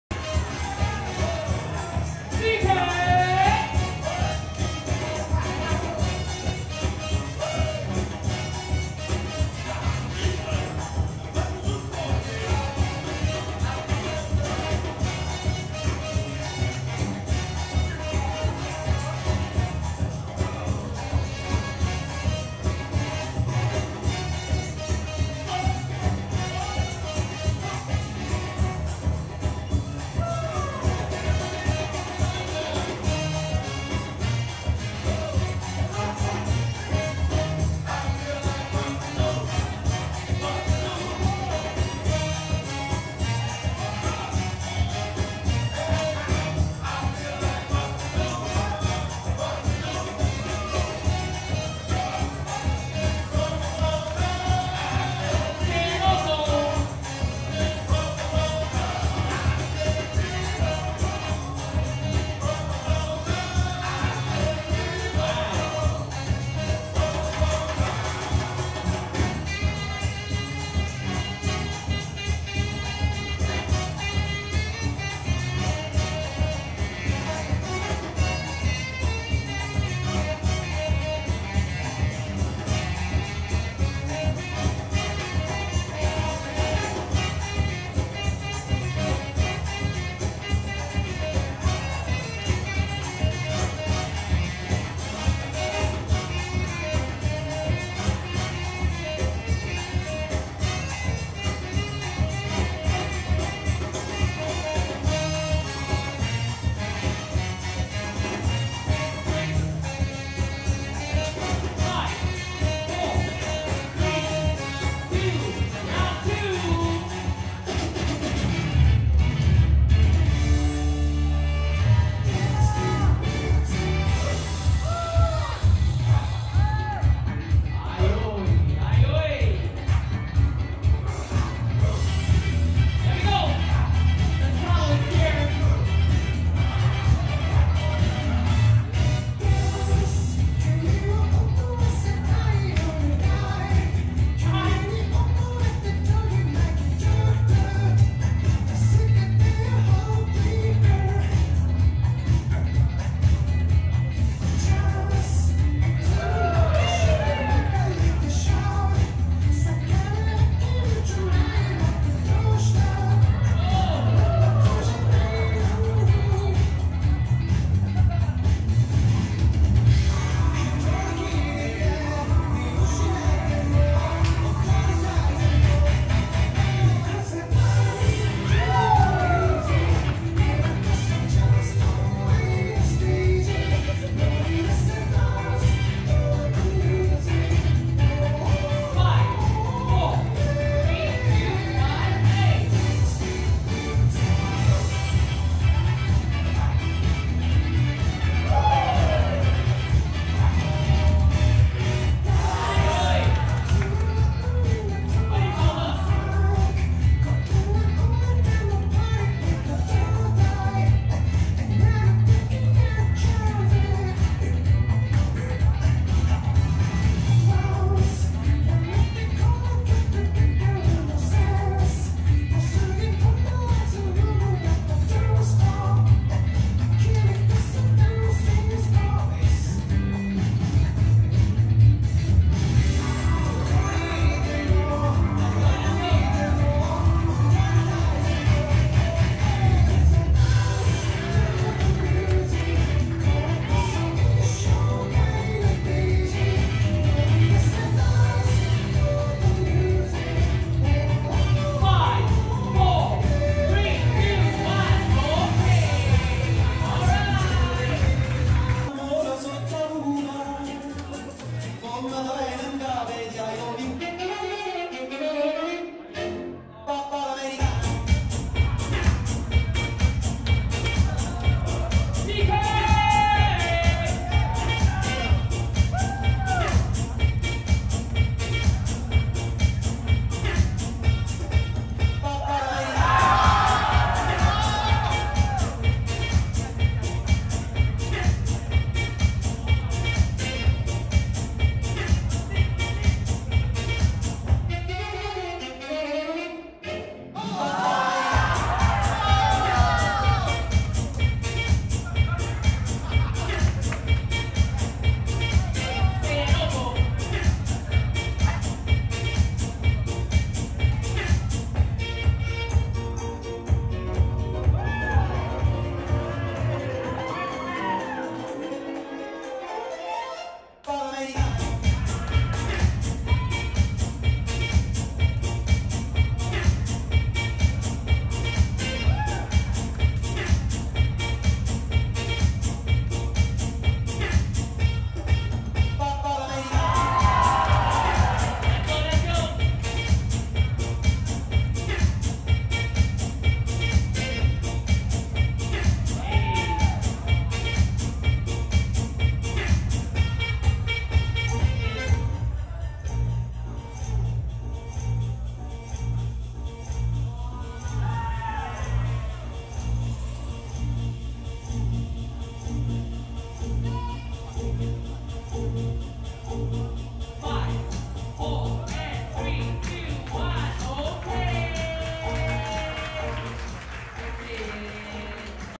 SSEI & OHCO VS FUNK TROUBLE | LOCKING TOP 8 | NTU Funk Jam 2022
NTU Funk Jam 2022

Locking Judges:
- LockerZee (KR)
- Lady Bird (SG)

Popping Judges:
- Eun.G (KR)
- Marzipan (SG)

DJ: G.Mak
Emcee: JIN

Filmed by: NTU CAC MIT
Organised by: NTU Soul Funky
Programme Partner: SCAPE